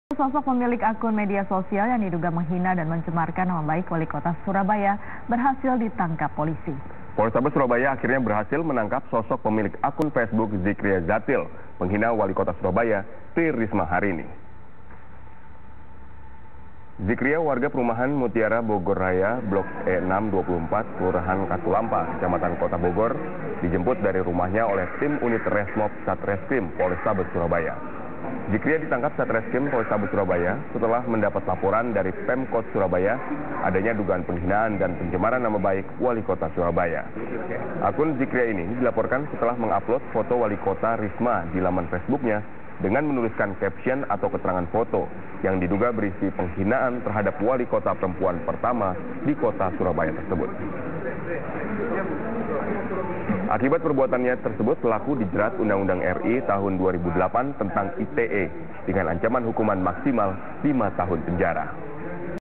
DUGAAN MENGHINA WALIKOTA Surabaya Tri rismaharini yang berujung penjara
Pemilik akun media sosial yang diduga menghina dan mencemarkan nama baik walikota Surabaya berhasil ditangkap polisi berhasil menangkap sosok pemilik akun Facebook penghina walikota Surabaya Tri rismaharini hingga perumahan Mutiara Bogor Raya Blok 624 Kelurahan Batu Ampar Kecamatan Kota Bogor dijemput dari rumahnya oleh tim unit Resmob satreskrim Polrestabes Surabaya